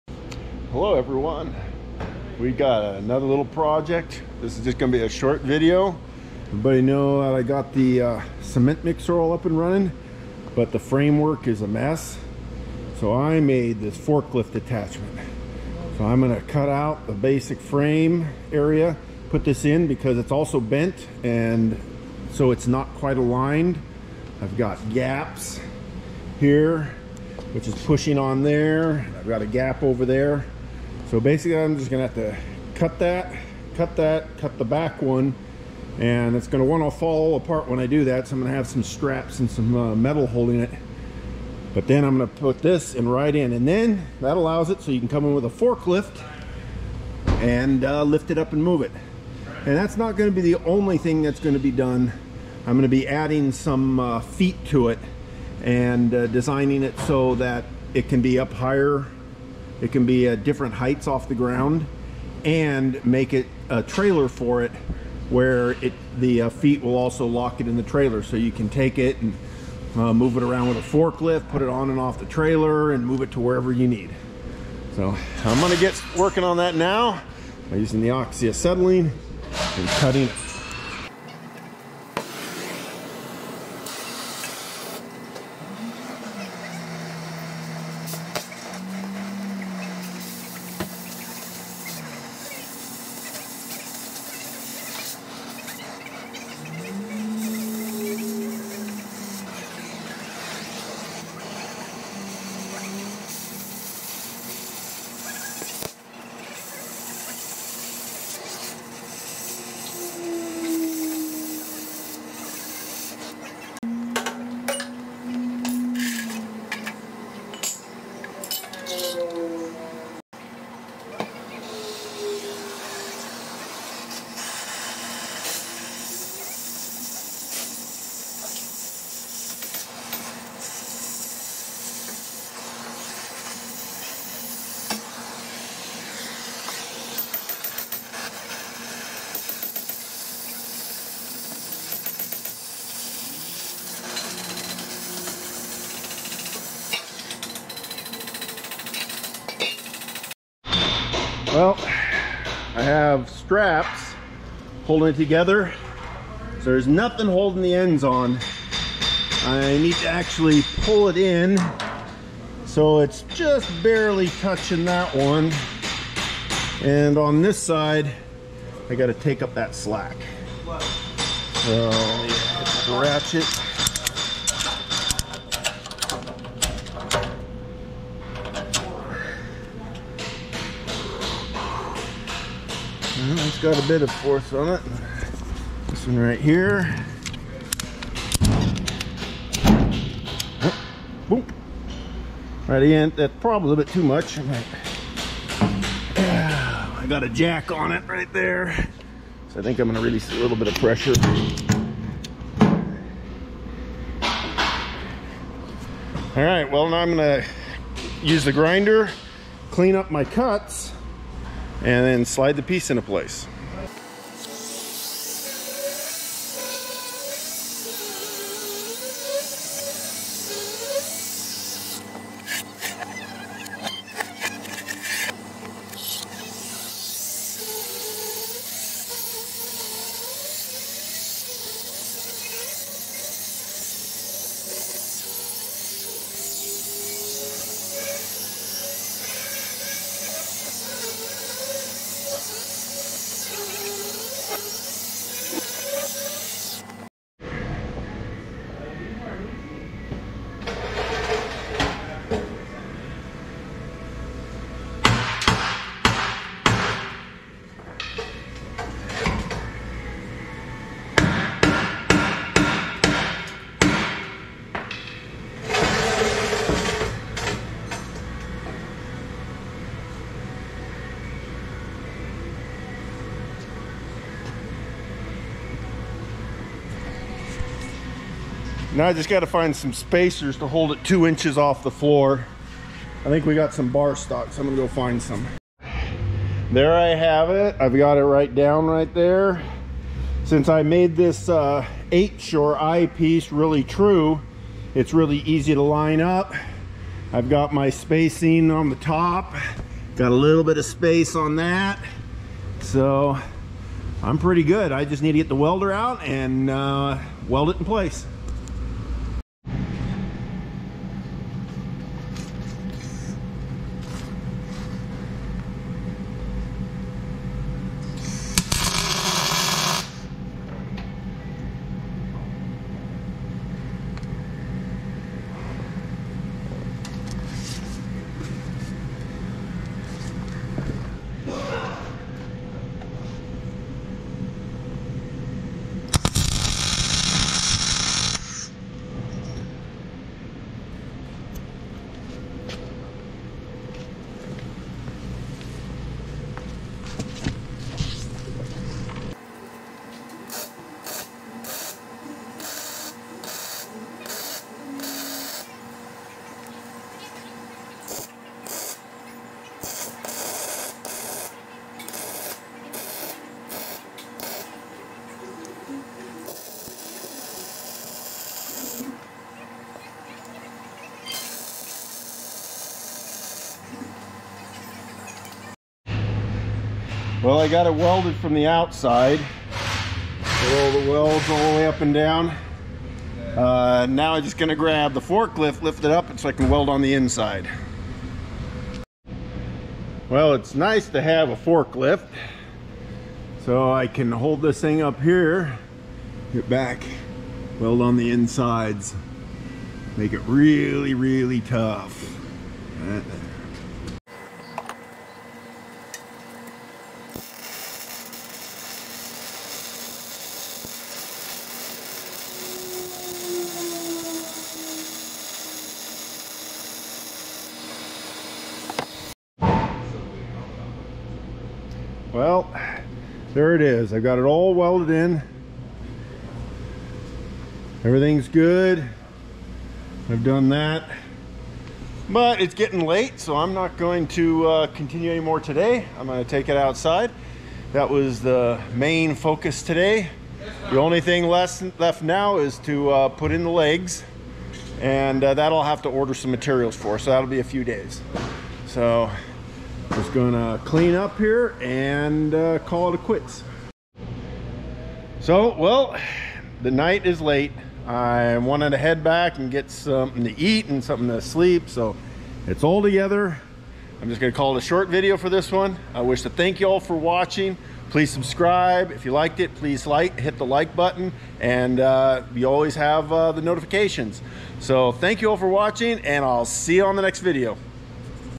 Cement Mixer (STONE 95CMED) forklift Upgrade
Short video of me upgrading the old STONE 95CMED with forklift lift points.